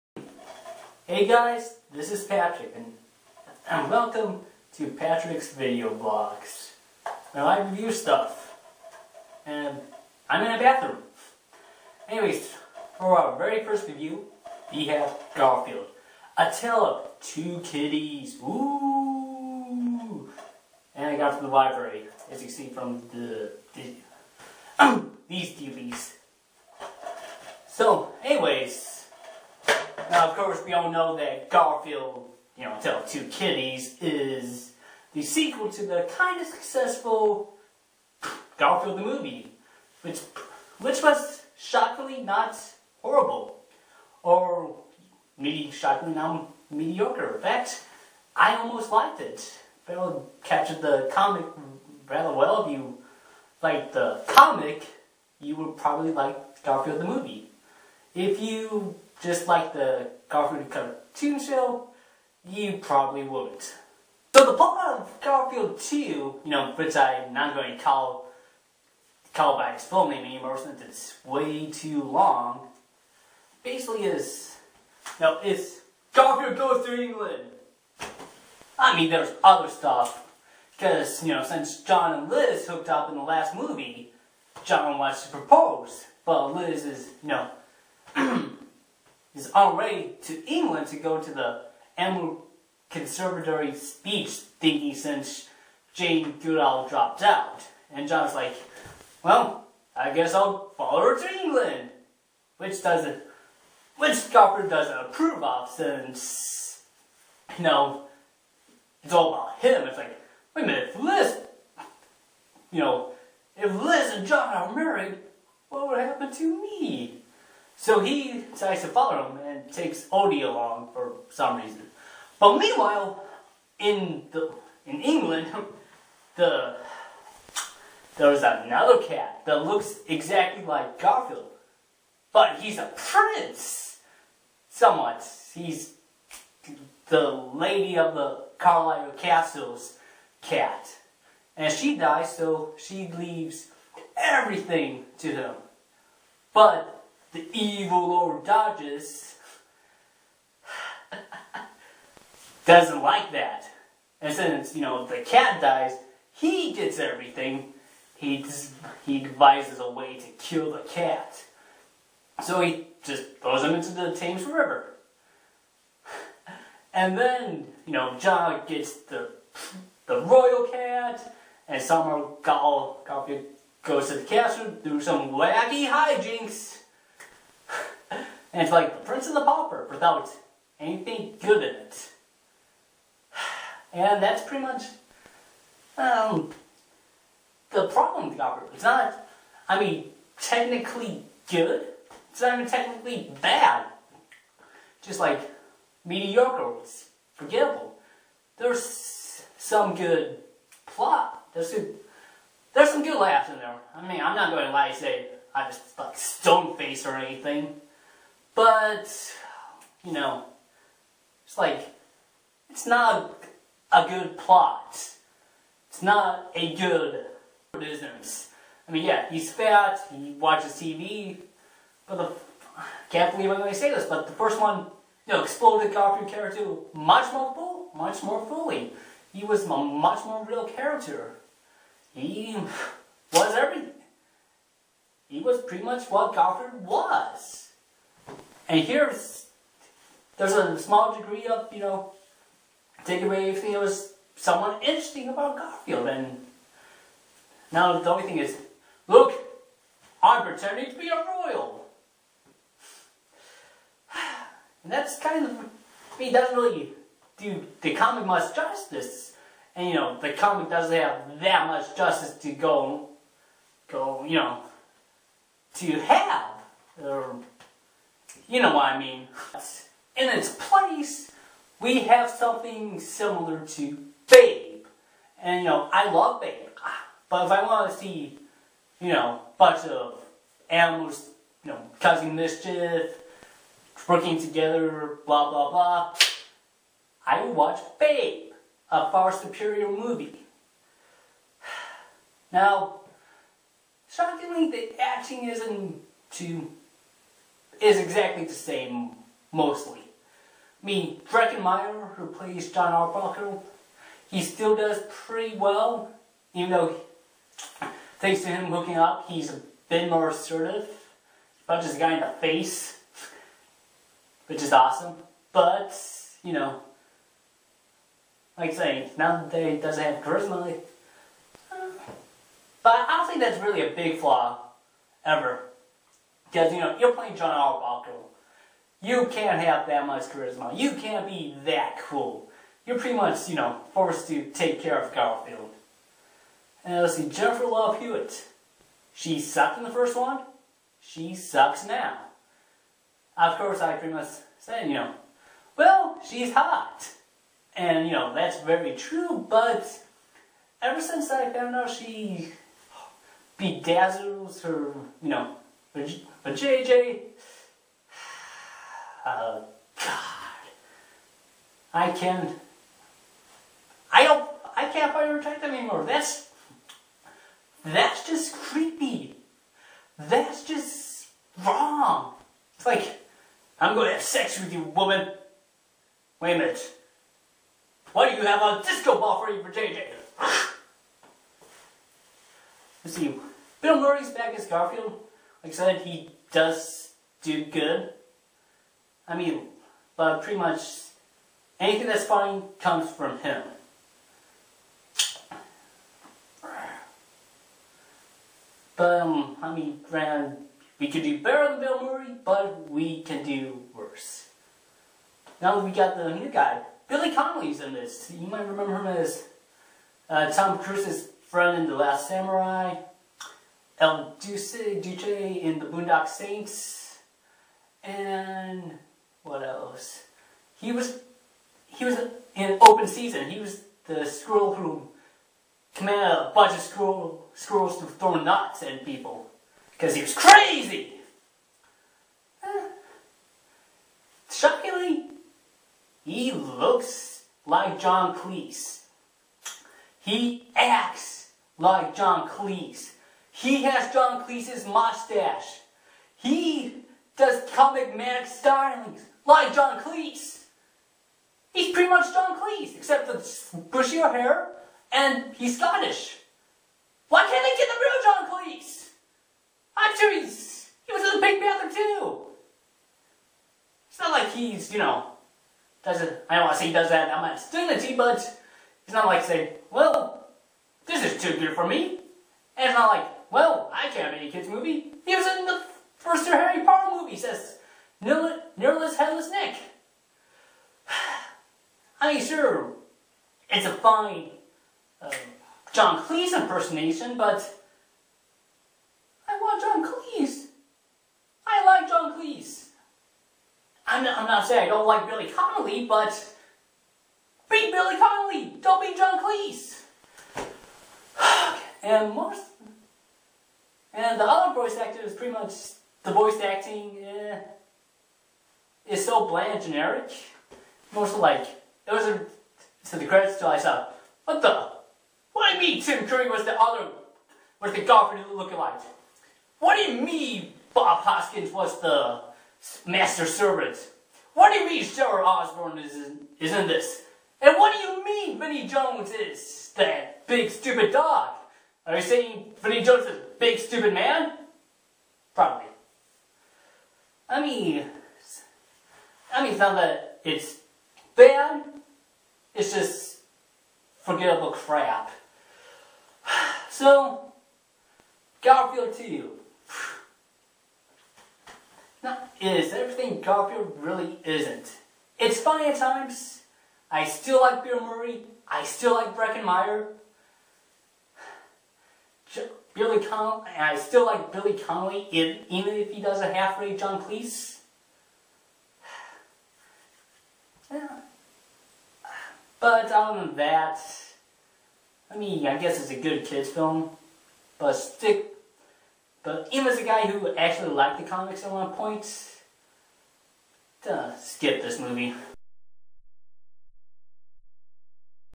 Garfield: A Tale of Two Kitties reviewed
My review of Garfield: A Tale of Two Kitties.